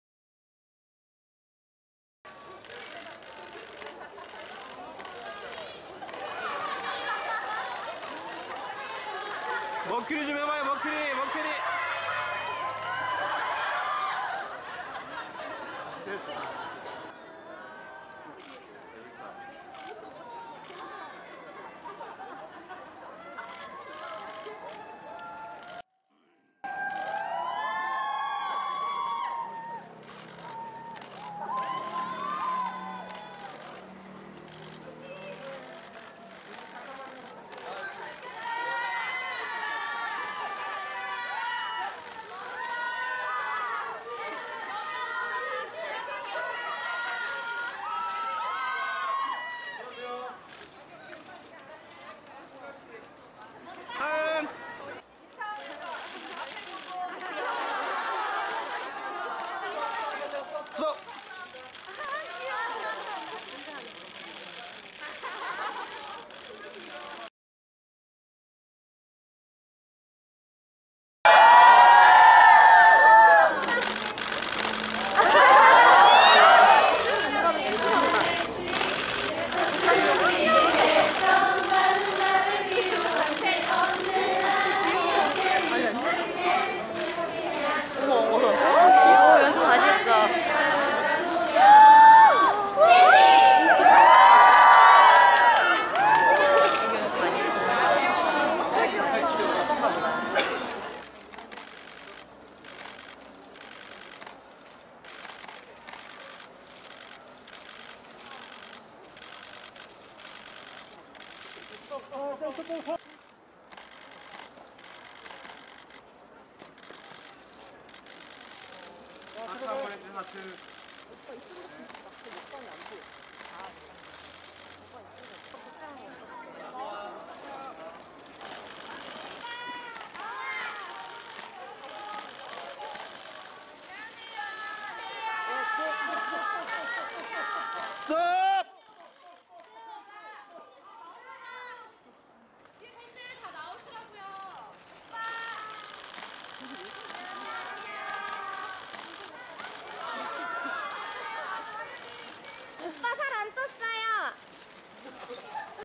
181222 HOT 춤을 커버하는 나하은, 토니안, 프레드 무큐리 KBS '해피투게더4' 출근길 직캠
12월 22일 오후 서울시 영등포구 여의도동 KBS별관에서 '해피투게더4' 녹화가 진행됐다. 이날 녹화는 연말 특집으로 문희준, 토니안, 황보, 샘김, 폴킴, 나은(에이프릴), 특별출연으로 나하은과 MC 유재석, 전현무, 조세호, 조윤희가 출연한다.